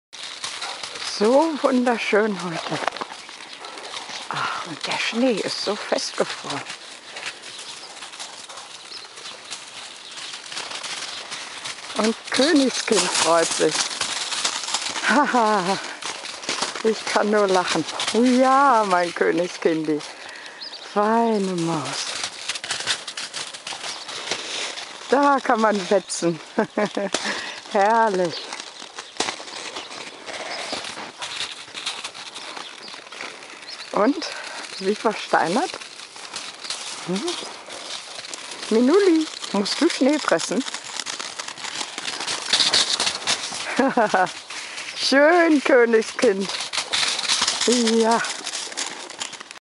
LIBERTE Danes enjoying the wonderful morning being at home again
LIBERTE: Baby, Himbi, Königskind, Kirschblüte, Himbi